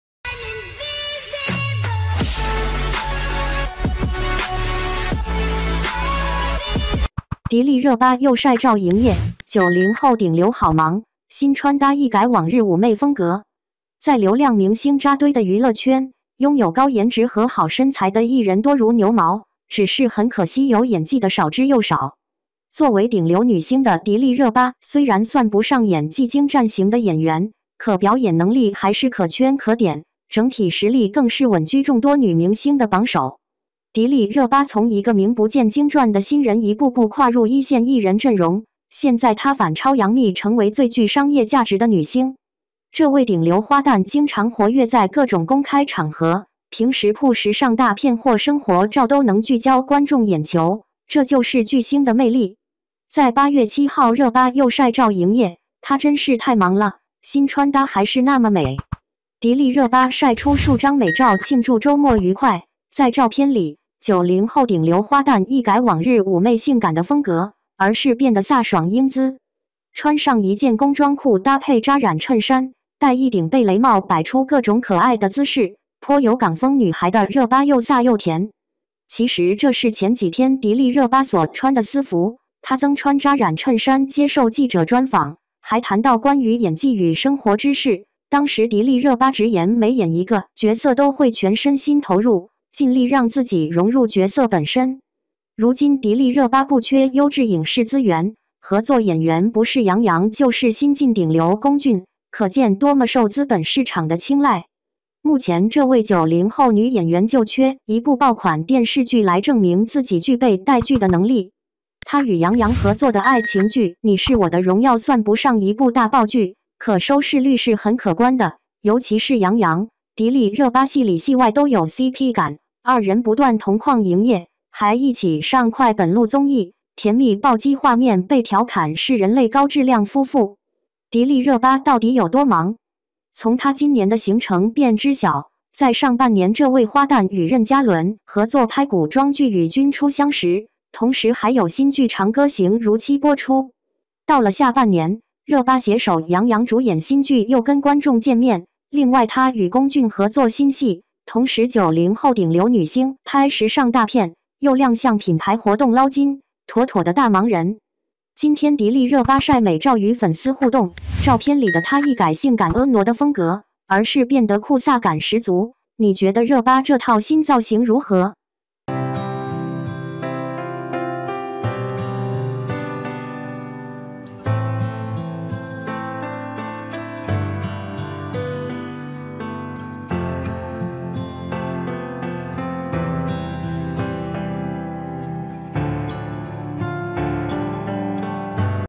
迪丽热巴又晒照营业，90后顶流好忙，新穿搭一改往日妩媚风格
迪丽热巴又晒照营业，90后顶流好忙，新穿搭一改往日妩媚风格 | 明星新闻 MKN Star News.